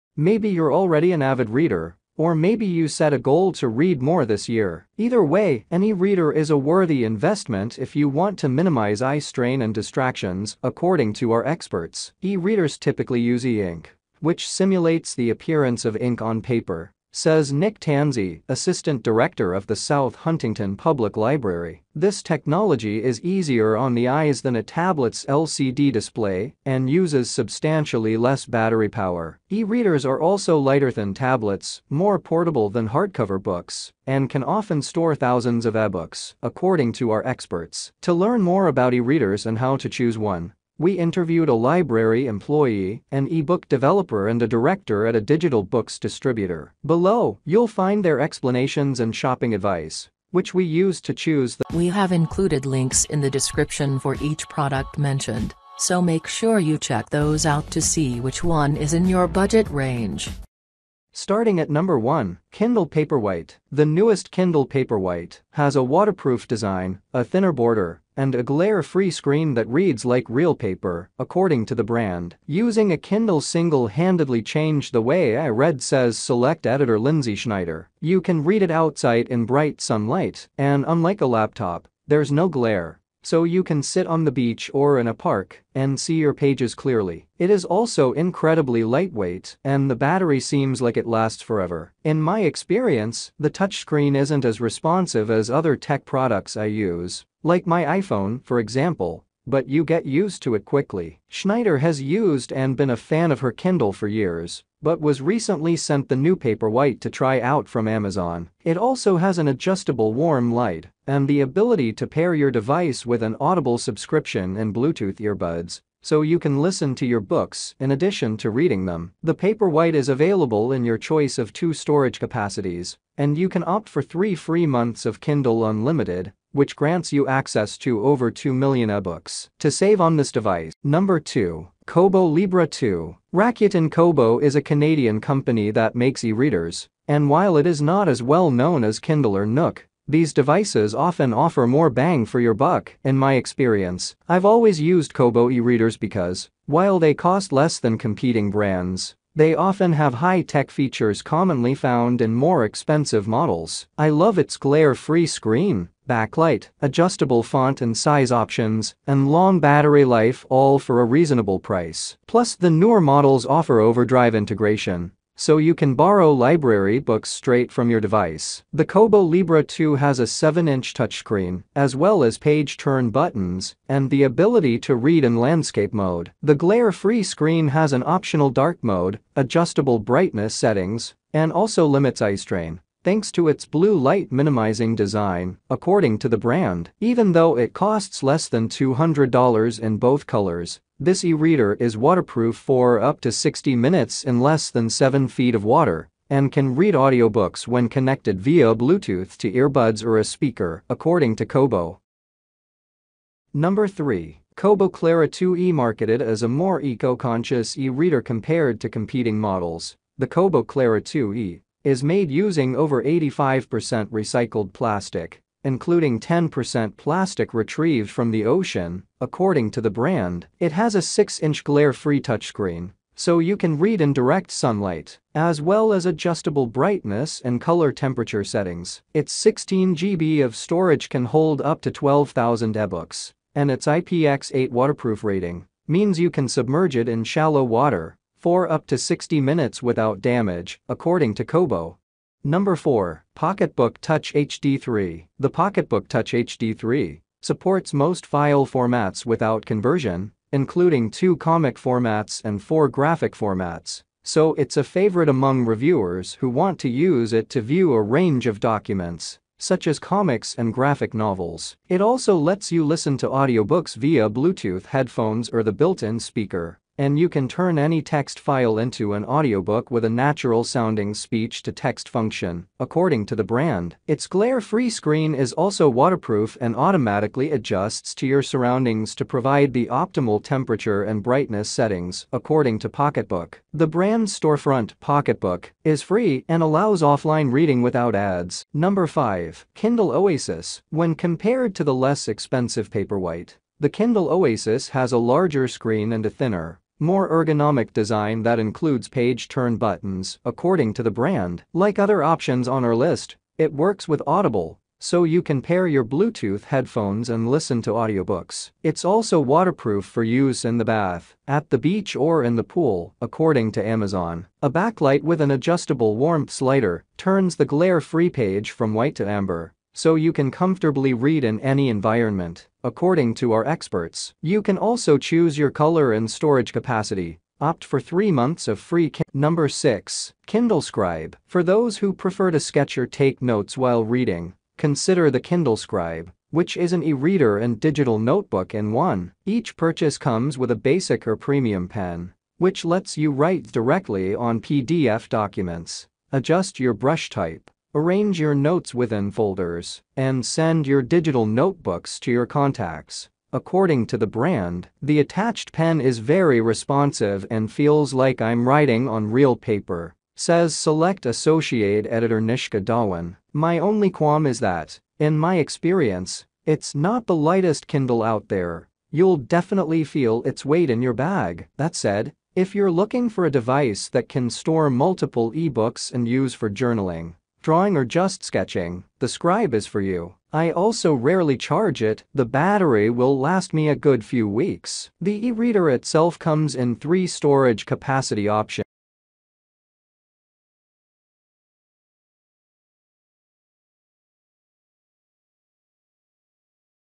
best e-readers of 2023, according to experts .you can buy
~ Product Link ~

1. Kindle Paperwhite:

2. Kobo Libra 2:

3. Kobo Clara 2E:

4. PocketBook Touch HD 3:

5. Kindle Oasis:

6. Kindle Scribe: